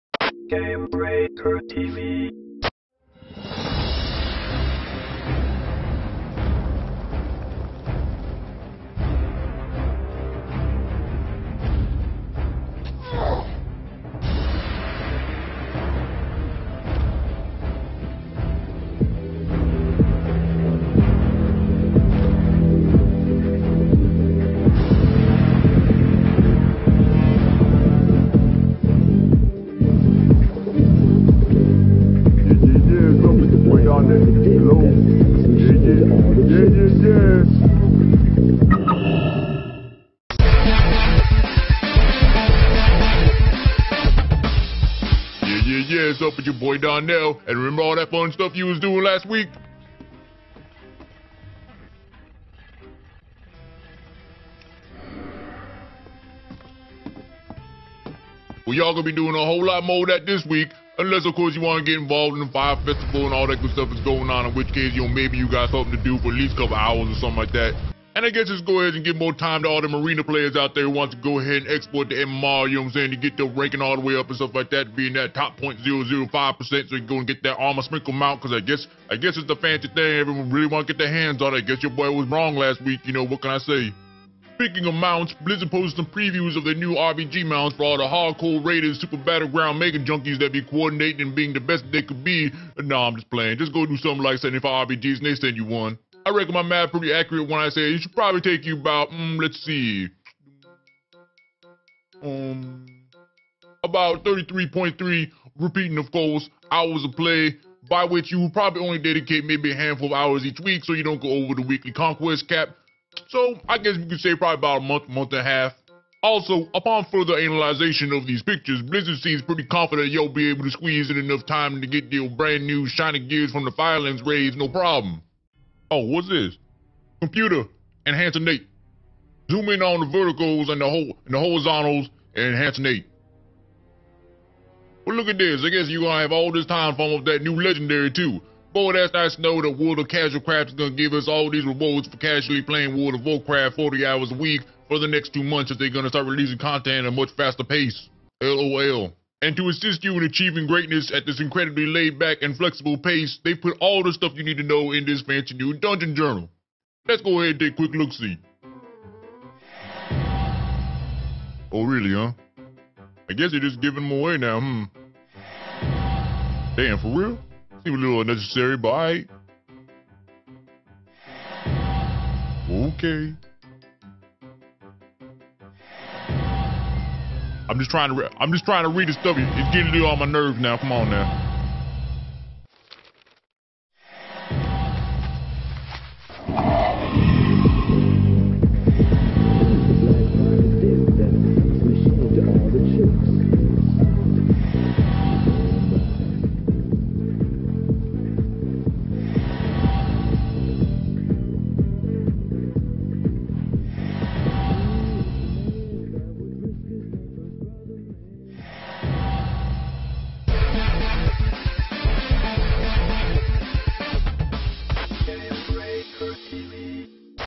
Blizz Blues 20: Enhancinate
This week on Blizz Blues, Darnell makes a few keen observations concerning a recent Blizzard announcement. He also throws some math at us and gets showered in completely necessary achievements.